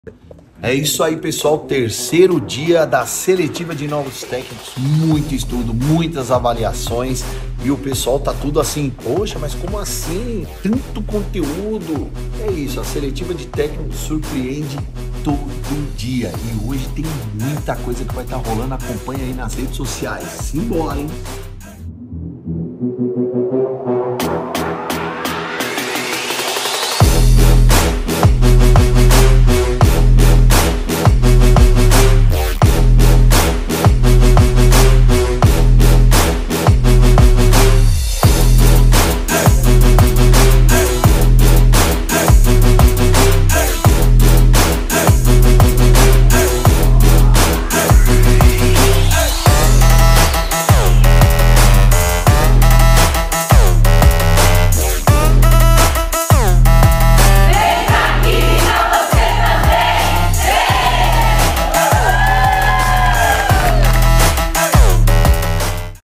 SELETIVA DE NOVOS TÉCNICOS - GARDEN HAIR - DIA 3
No nosso terceiro dia da Seletiva de Técnicos, tivemos uma aula de química que foi um verdadeiro show de conhecimento. Foi um dia repleto de aprendizado e descobertas, com foco nas ações químicas e suas aplicações. Deixe seu comentário abaixo com suas próprias dicas de cuidados com o cabelo ou sugestões para futuros vídeos.